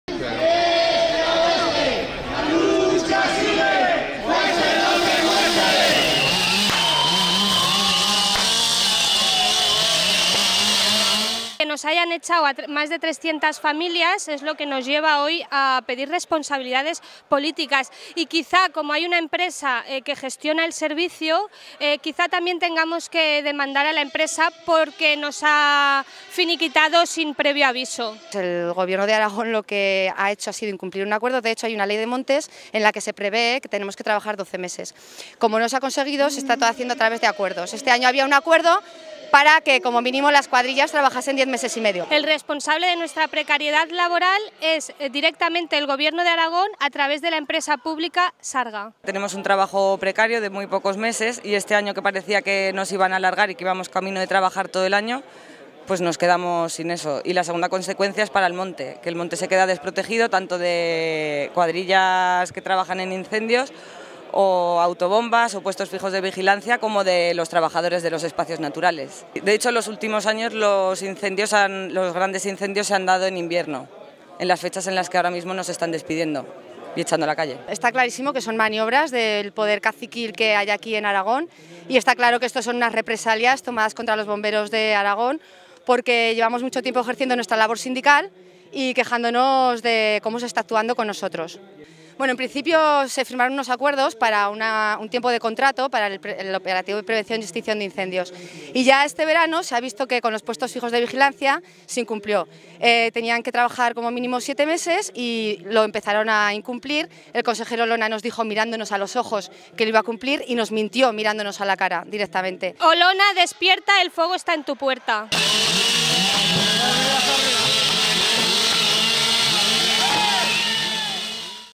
Concentración BomberosForestales de Aragón
Concentración frente a la sede del gobierno de Aragón en protesta por el incumplimiento de sus compromisos y el despido de trabajadores.